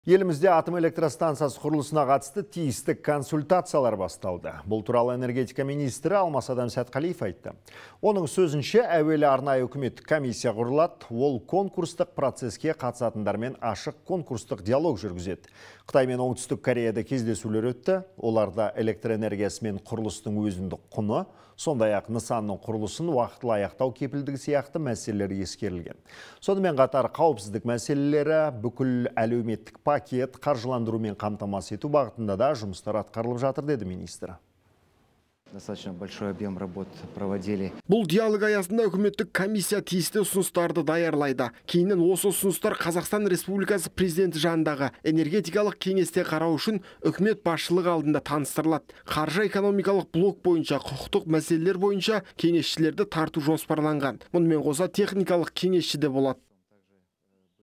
АЭС құрылысы бойынша үкіметтік комиссия құрылады | Jibek Joly news
Елімізде АЭС құрылысына қатысты тиісті консультациялар басталды. Бұл туралы Энергетика министрі Алмасадам Сәтқалиев айтты. Оның сөзінше, әуелі арнайы үкіметтік комиссия құрылады. Ол конкурстық процеске қатысатындармен ашық конкурстық диалог жүргізеді. Қытай мен Оңтүстік Кореяда кездесулер өтті. Оларда электр энергиясы мен құрылыстың өзіндік құны, сондай-ақ нысанның құрылысын уақытылы аяқтау кепілдігі сияқты мәселелер ескерілген. Сонымен қатар, қауіпсіздік мәселелері, бүкіл әлеуметтік пакет, қаржыландырумен қамтамасыз ету бағытында да жұмыстар атқарылып жатыр деді, министр.